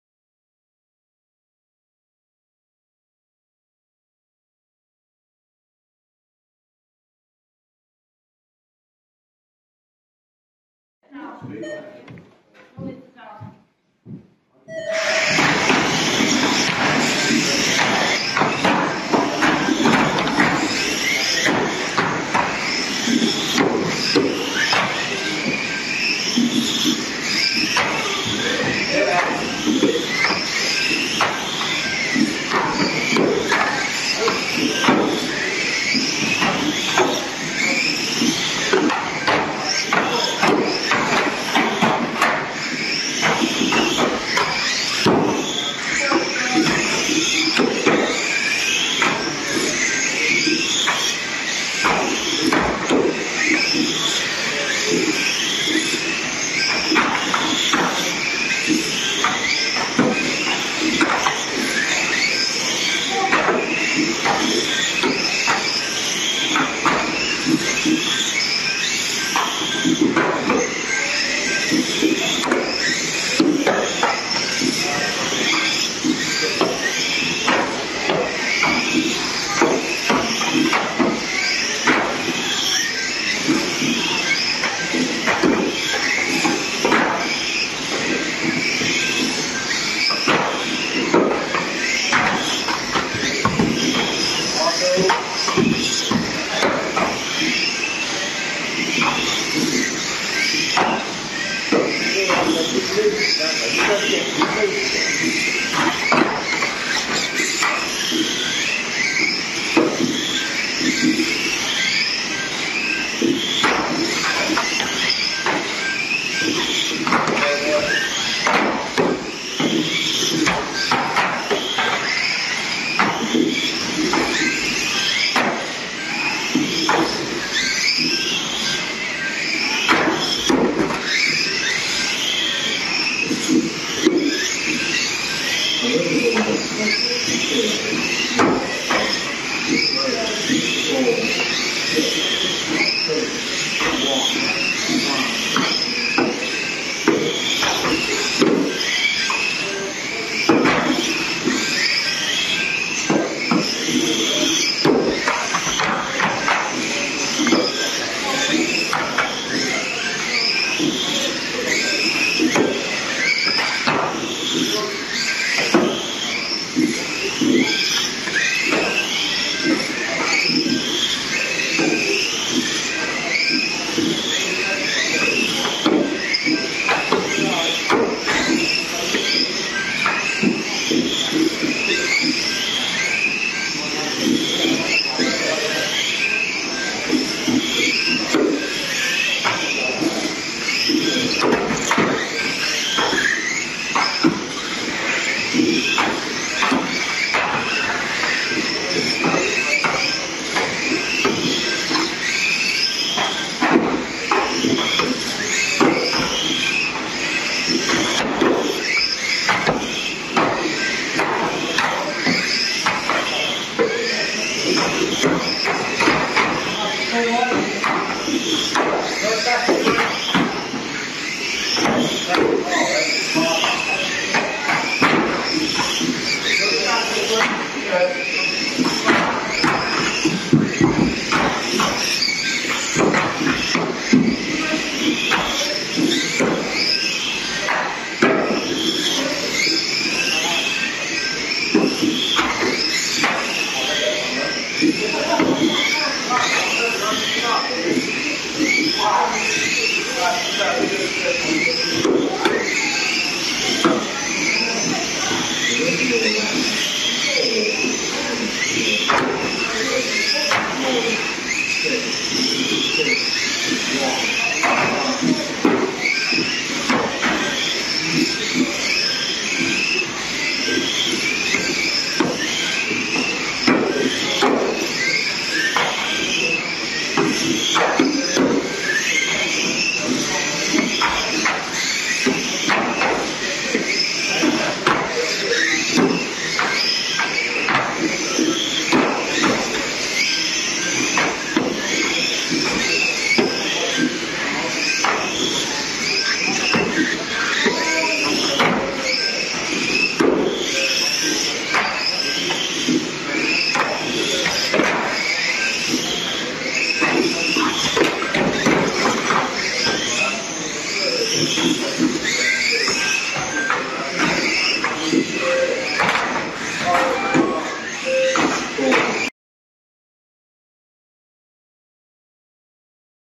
Hots Racers. 2 litre stock cars. Heat 3. 15/7/23
Heat 3 of the 2 litre stock cars at Hot Racers in Haywards Heath. 15th July 2023
St Wilfrid’s School, Eastern Road, Haywards Heath, West Sussex, RH16 3NL